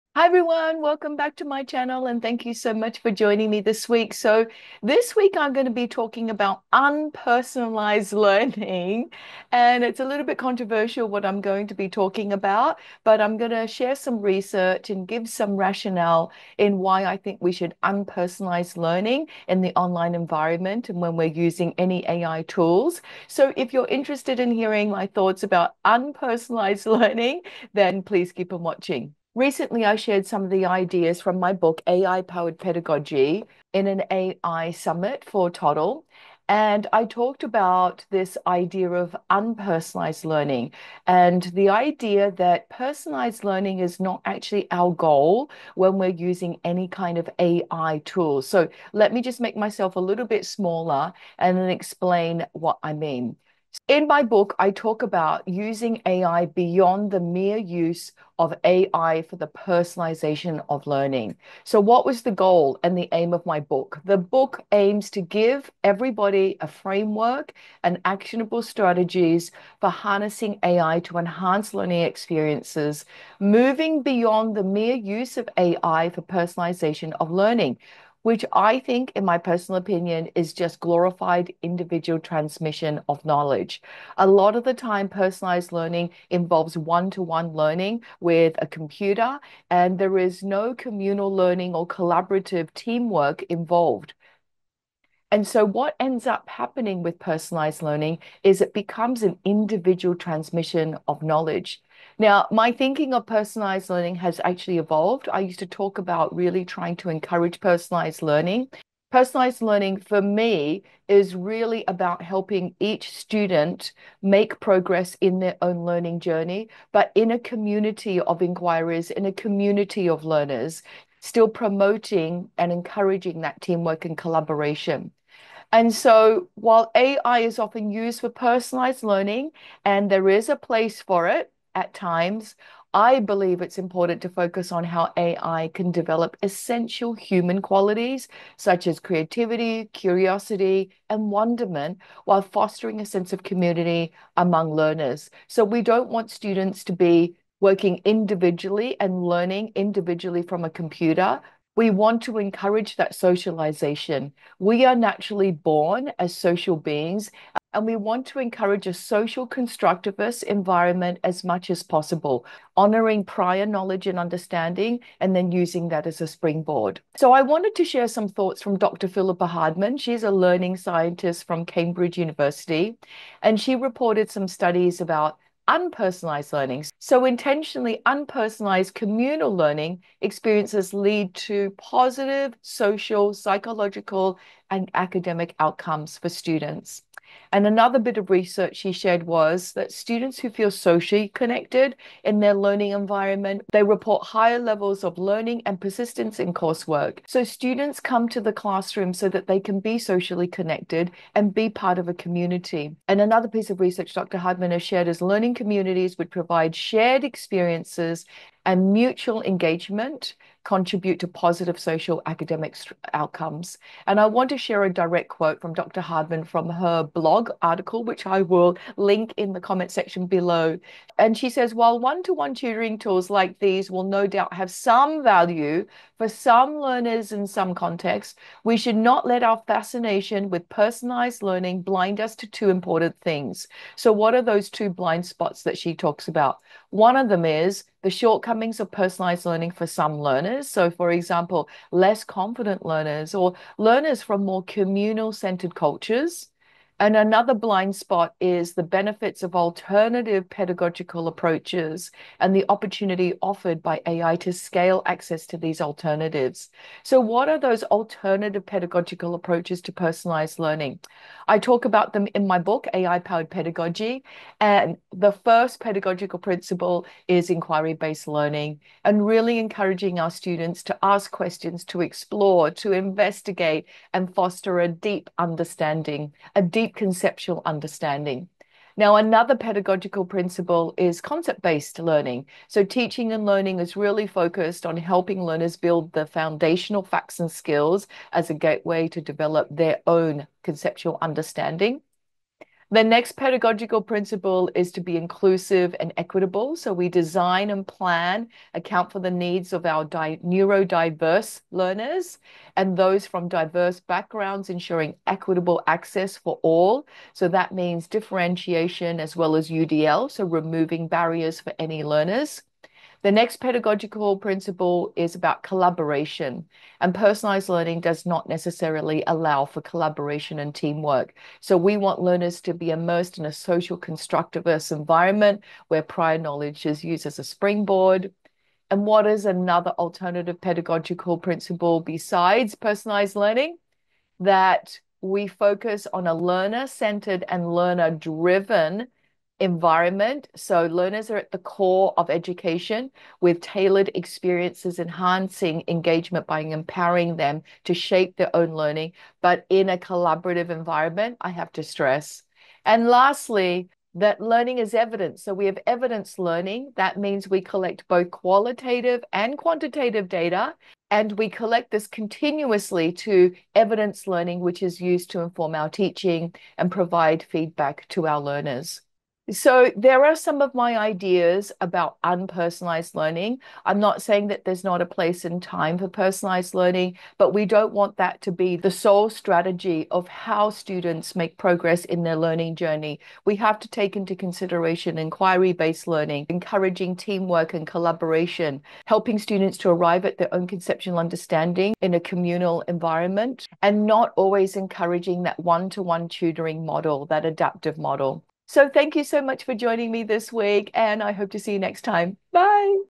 277. Unpersonalised Learning
277. Unpersonalised Learning With Jennifer Chang

Chapters:
0:00 hi everyone welcome back to my channel
1:01 what was the goal and the aim of my book
2:01 still promoting and encouraging that
3:02 social psychological and academic
4:02 confident Learners or Learners from more
5:01 Learners and those from diverse
6:24 consideration inquiry based learning